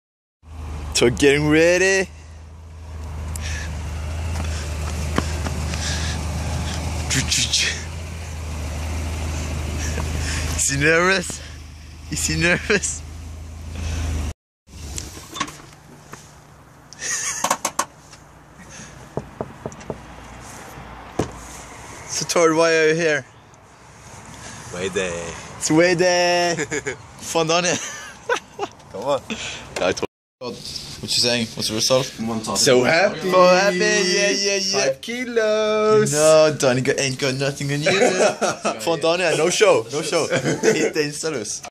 Summeriscoming vlog 1 Tord weigh in!
Fyste official weigh in day ette 6 uke med working on themselves! And dani fucking takes a no show?!

Tord showing good spirit!